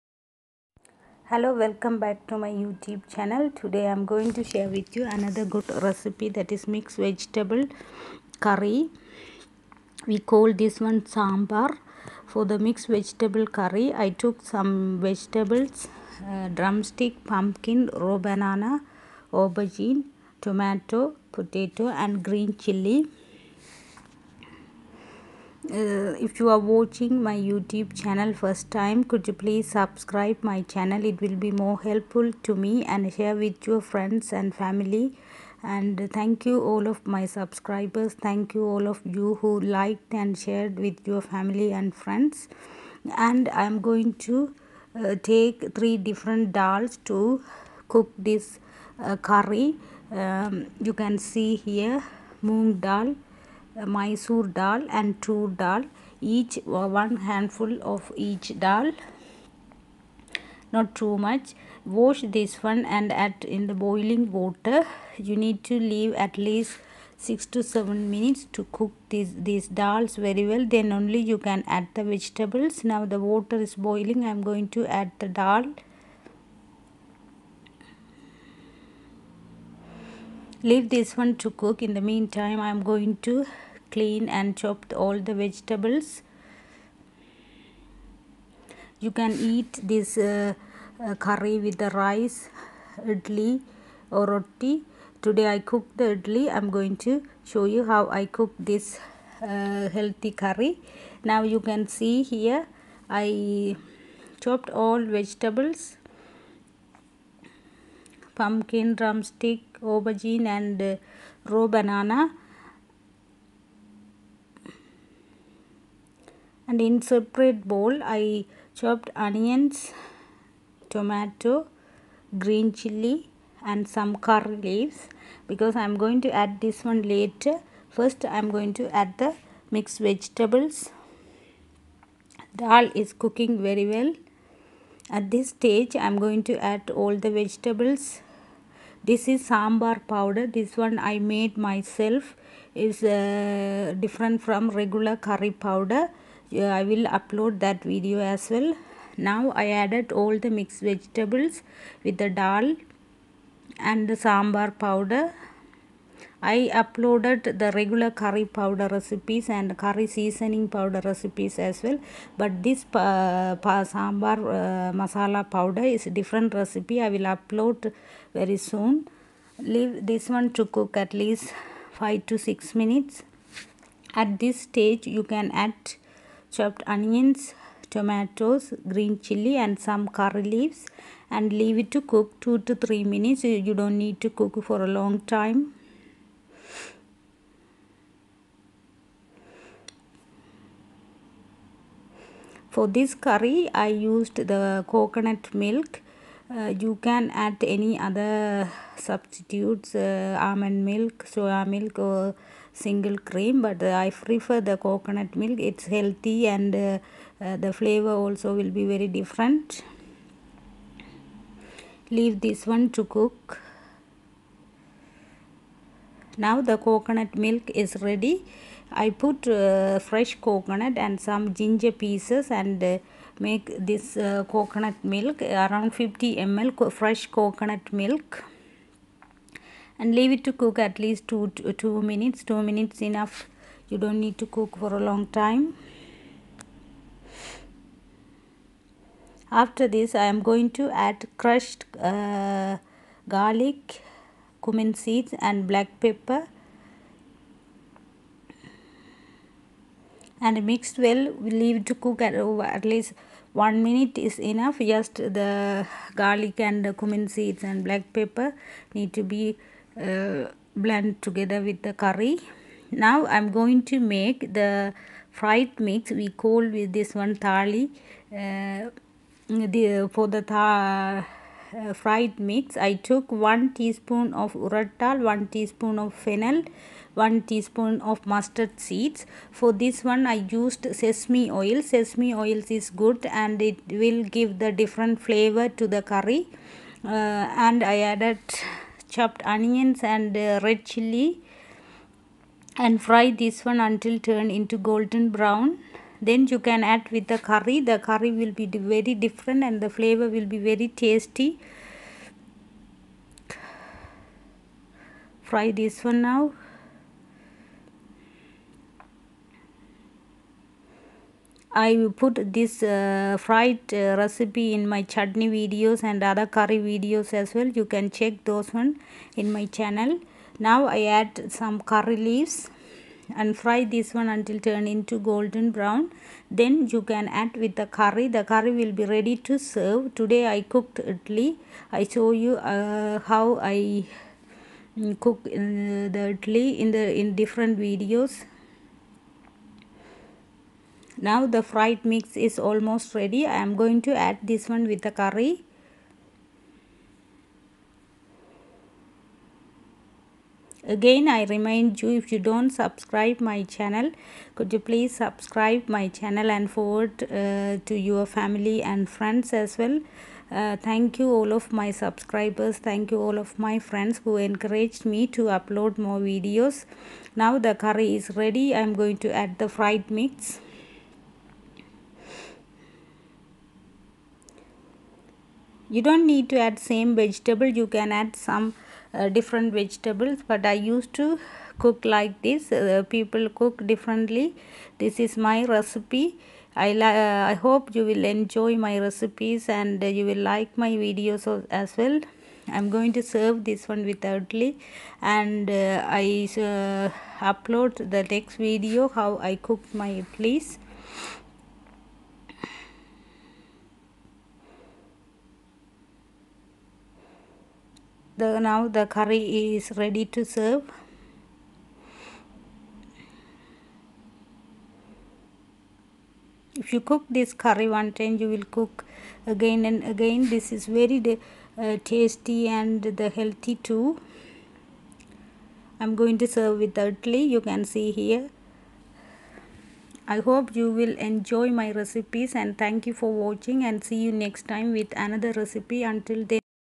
Mixed Vegetable Curry- Sambaar
You need any 5 vegetables
Dals -lentil, toor dal & moong dal
2 medium size onions
2 medium size tomatoes
Greenchillies
Curry leaves
Ginger & garlic paste
Sambar powder
Turmeric powder
Coconut milk /almond milk
Boil the water in a pan & add the dals
After cooking the dals,add all mixed vegetables
Add some sambaar powder
After some time (around 5 to7 minutes),add the diced oinions,green chillies,tomatoes & curry leaves.